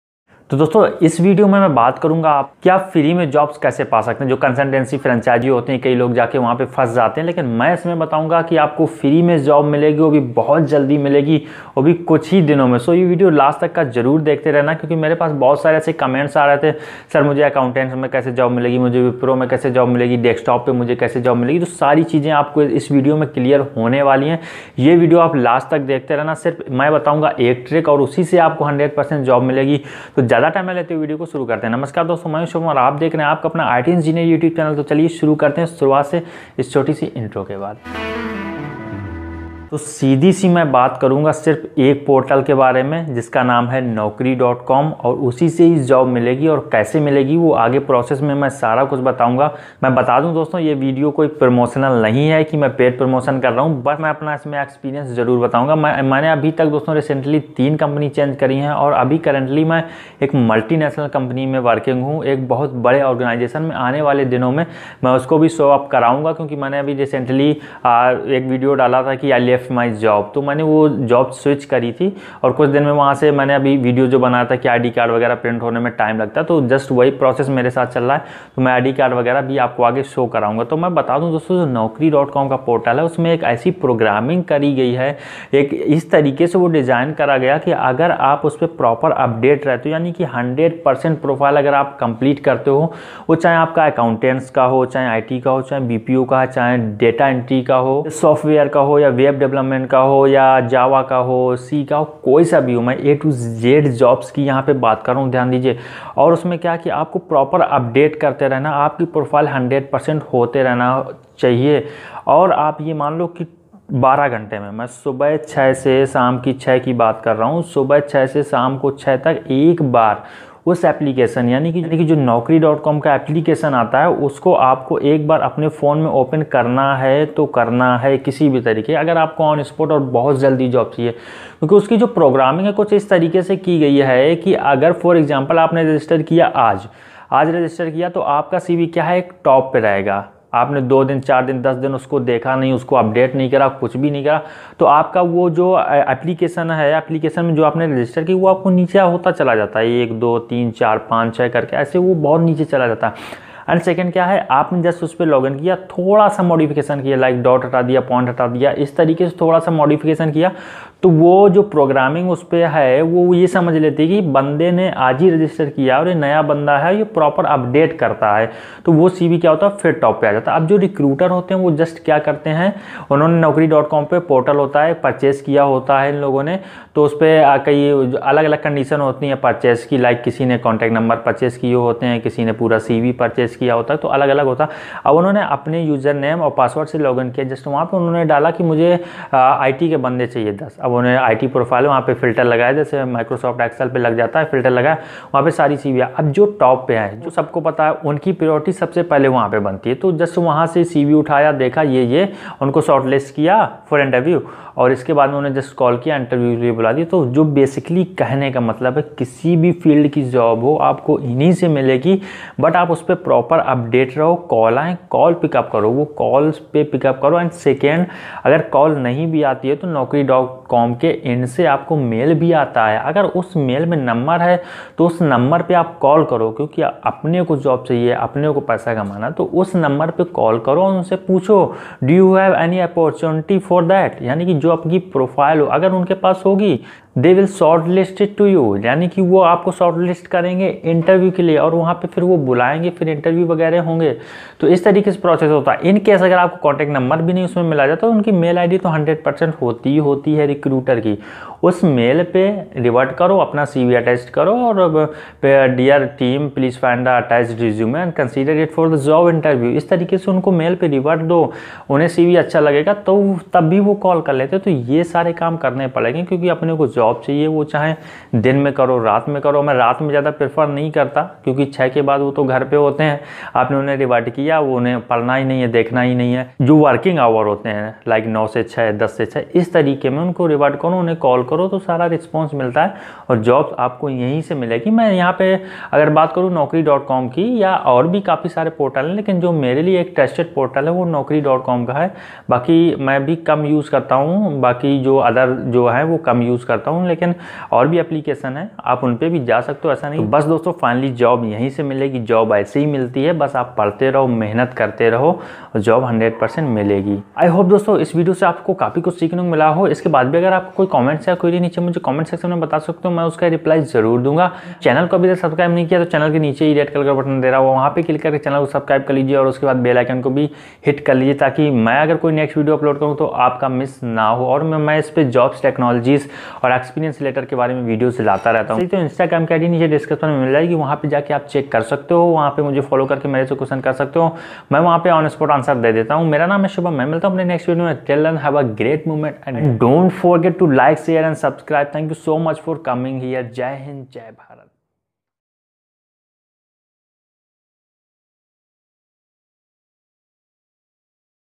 Only One Master Trick ? How To Get Free Job In Only 3 Days 100% Guarantee ? ये Trick कोई नहीं जानता.
Hi Friends .

Maine is video me btaya h ki free me naukari kaise milti h vo bhi sirf 1 trick se isme btaunga naukari portal ke bare me jo ki aapki bahut jyada help karega ,,,,,Thanks.,,,please subscribe , like and share ,,,

" Techs Update on daily basis..."

Thanks.